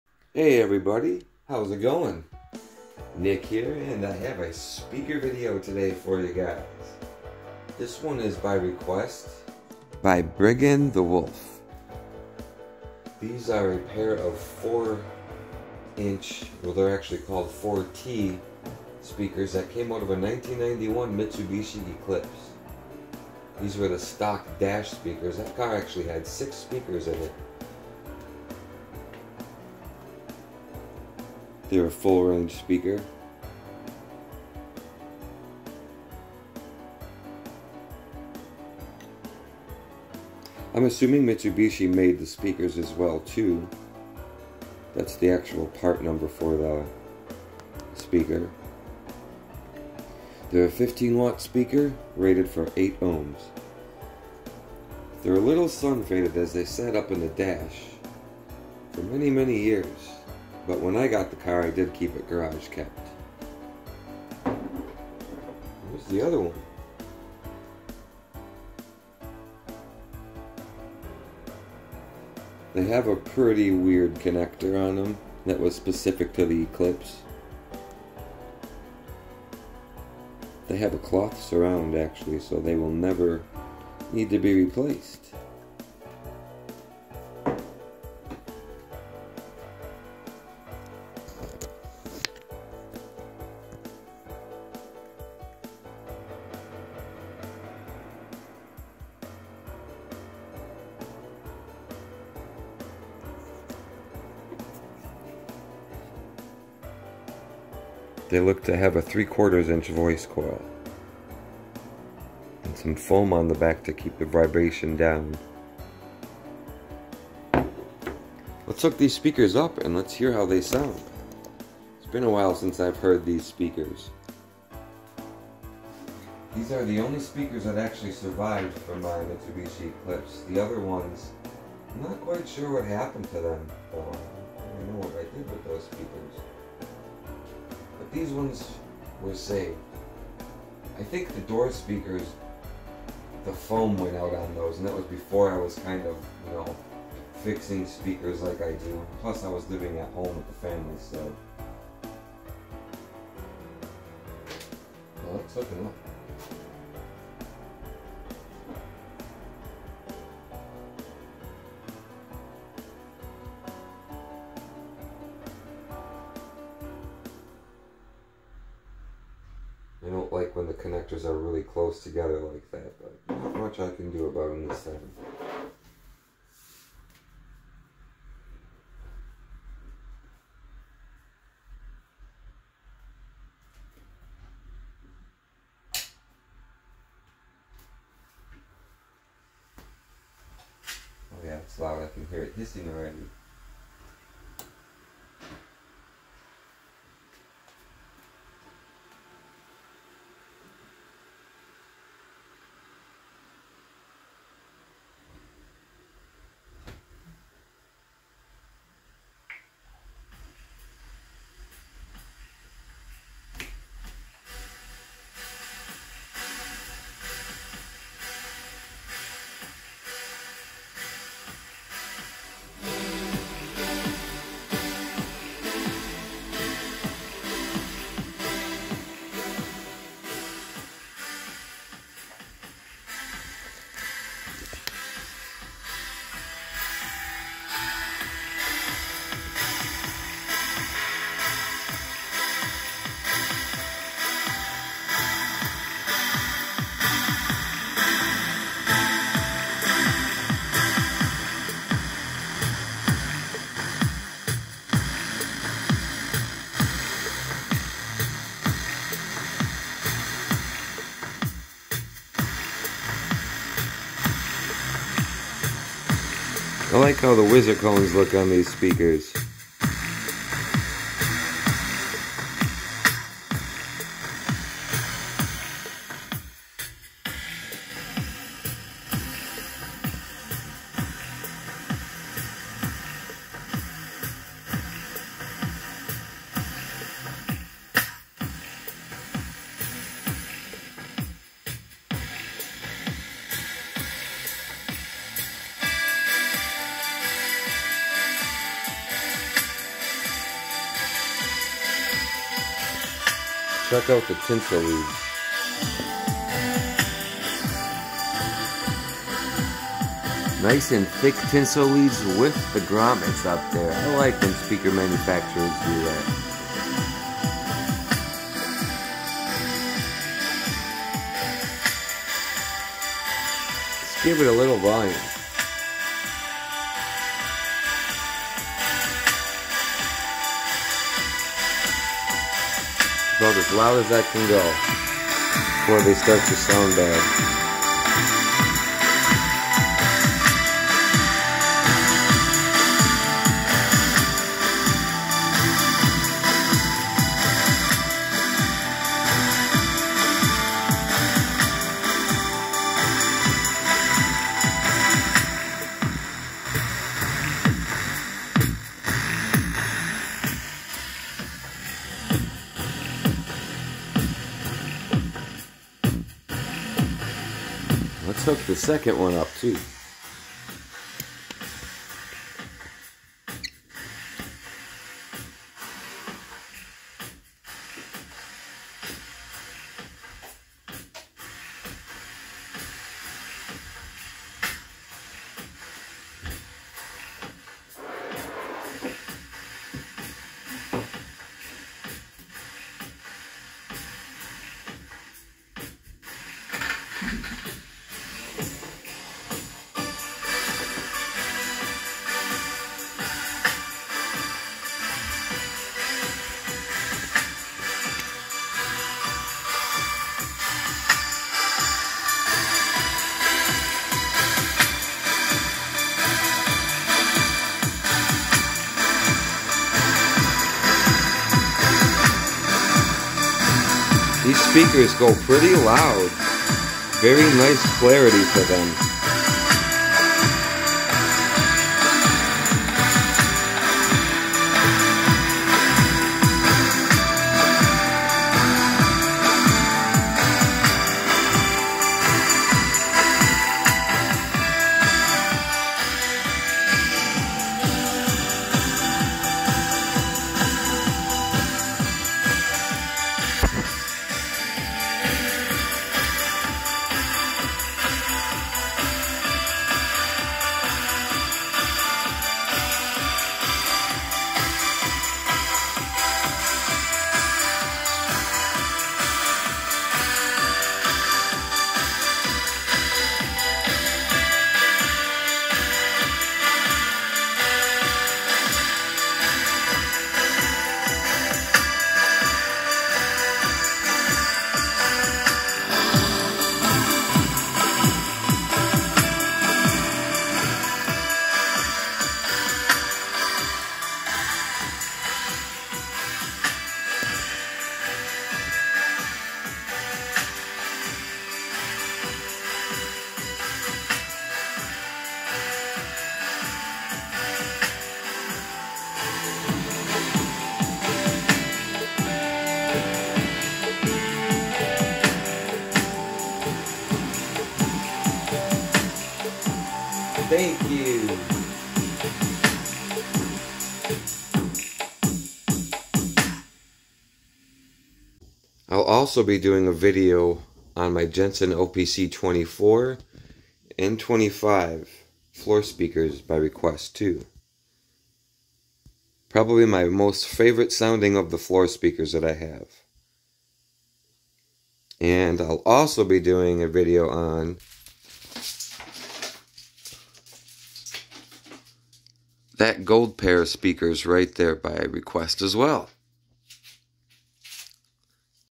4" Mitsubishi eclipse dash speakers from 1991
This is a pair of 1991 Mitsubishi eclipse GSX dash speaker's. I owned this car and replaced all it's speaker's. Speaker's work perfect and are in great shape.